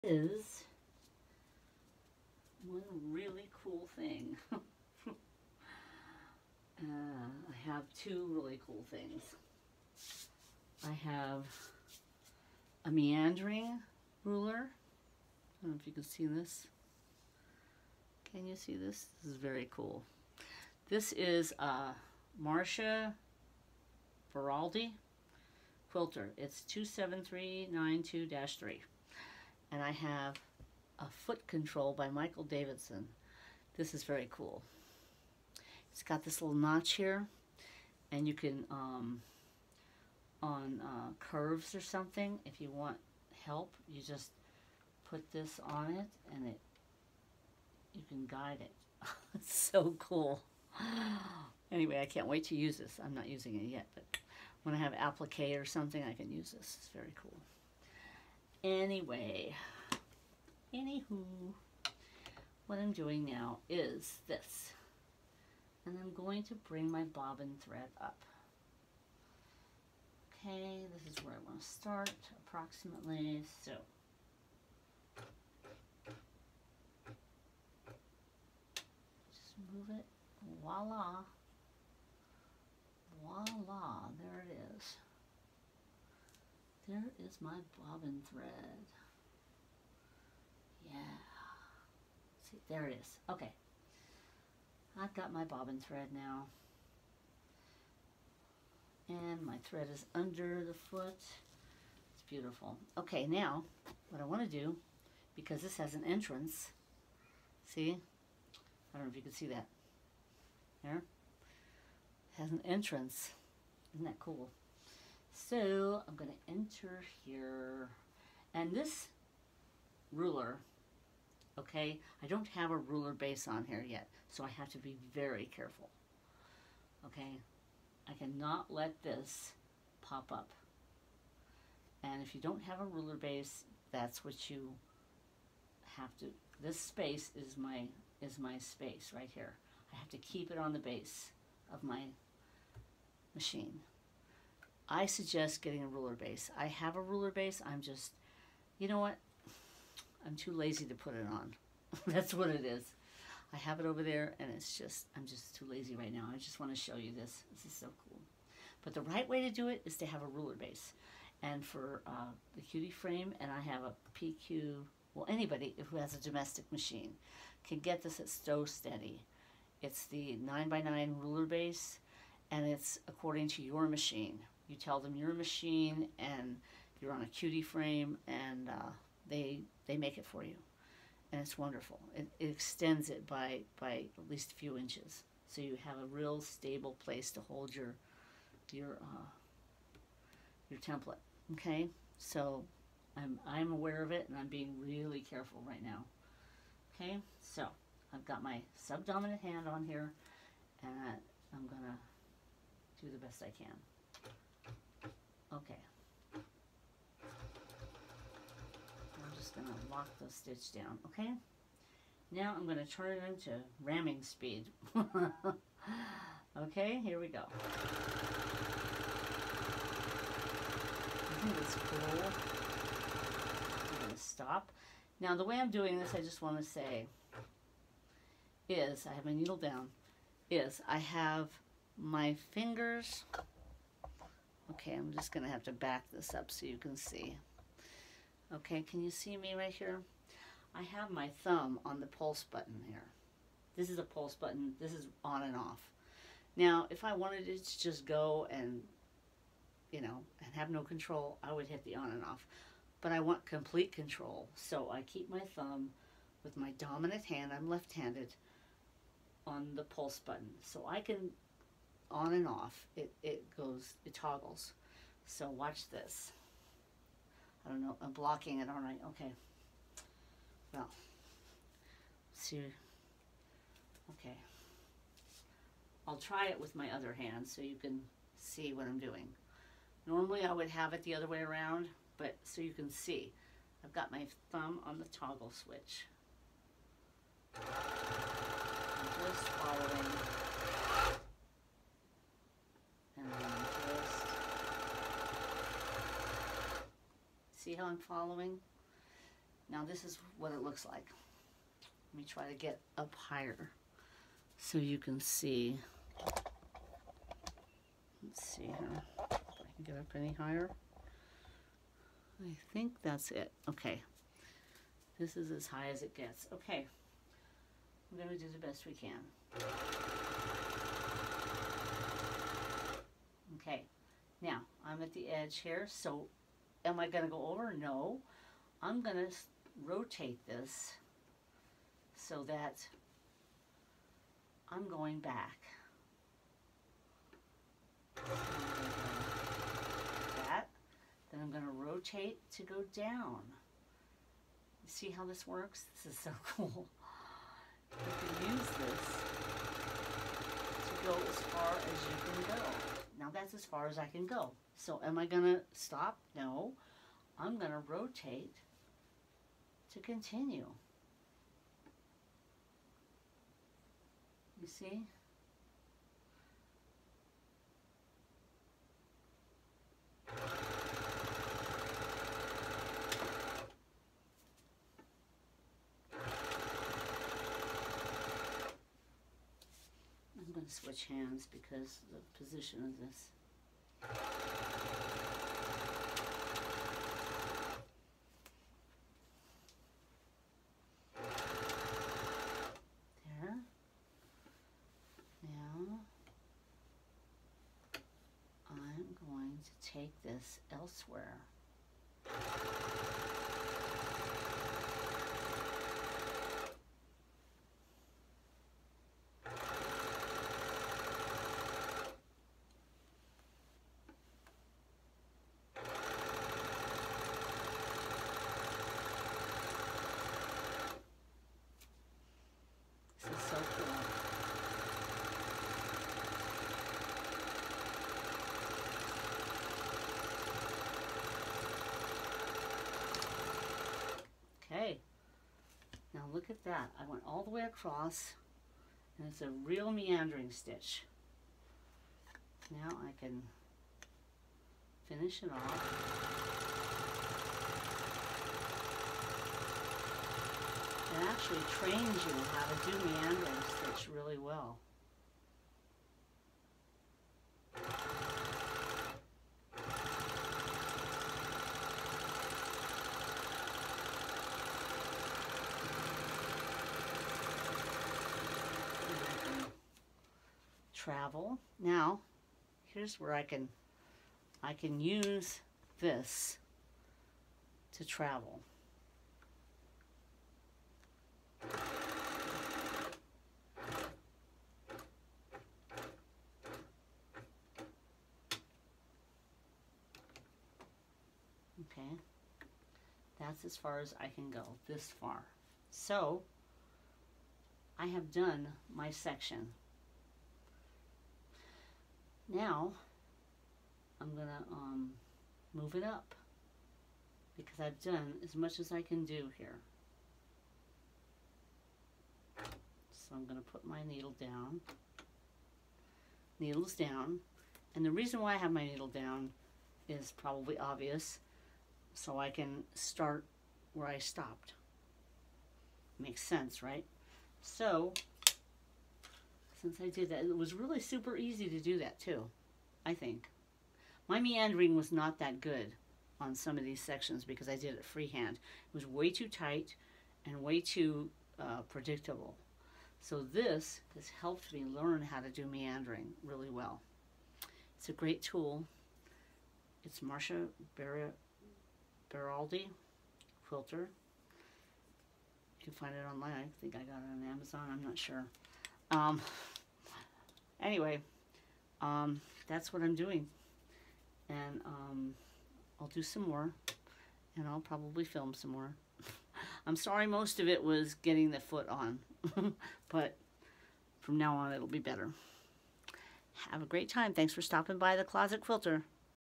Using a meandering ruler template on the Cutie
The Closet Quilter using a meandering ruler template on the Cutie Frame.
LINKS :
Marsha Baraldi meander ruler  template used

Grace co Speed Controller  used on handles instead of foot pedal